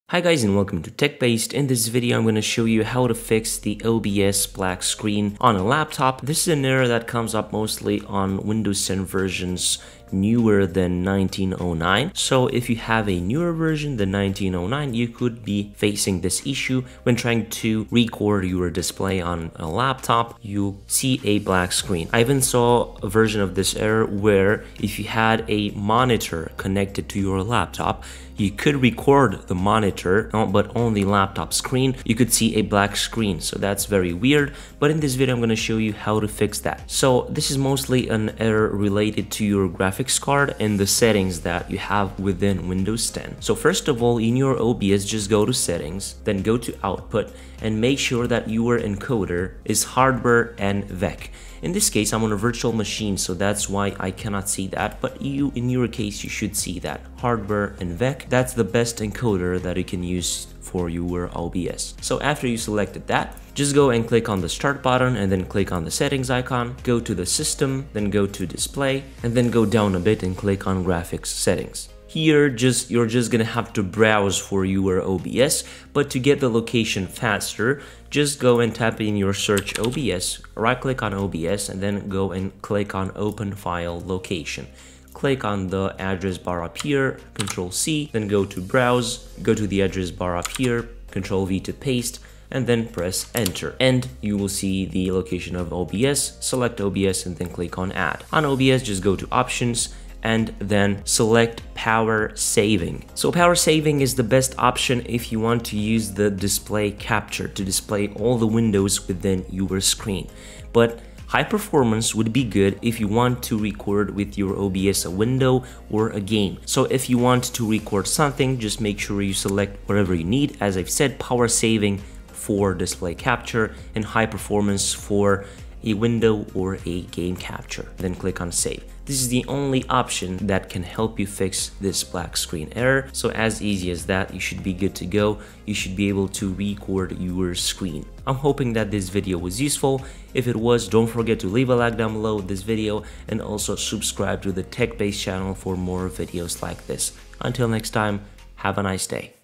Fix OBS Black Screen on a Laptop (2021)
In this video I will show you how to Fix OBS Black Screen on a Laptop (2021).